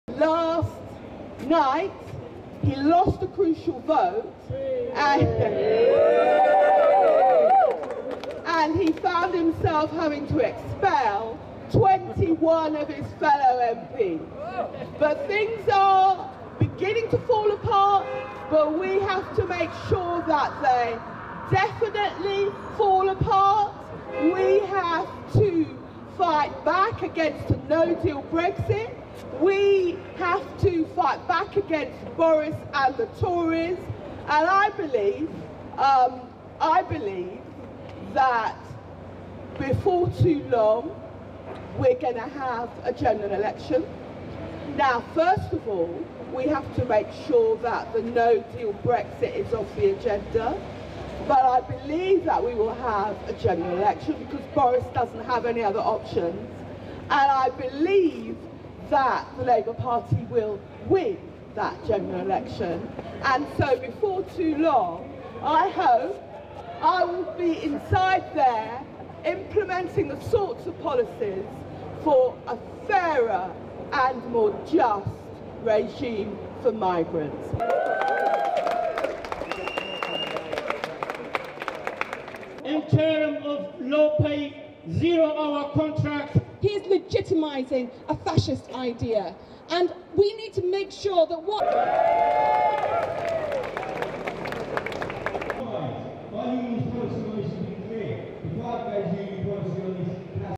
Diane Abbott to Implement Fairer Regime for Migrants
Shadow Home Secretary Diane Abbott calls for General Election and says she will implement a fairer regime for migrants at an anti-Brexit rally outside the Home Office, London, UK. 04/09/2019.

Terms
These video are for viewing only and may not be embedded or otherwise published without permission.